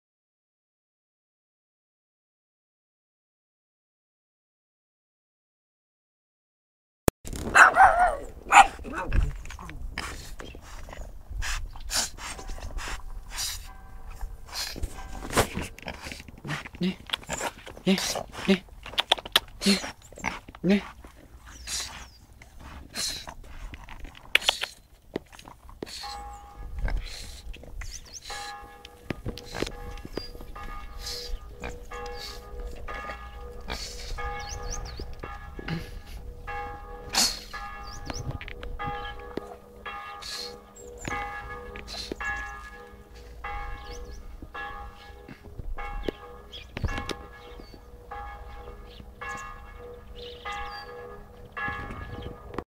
Une rencontre insolite
une rencontre insolite sur le bord de la route lors de ma tournée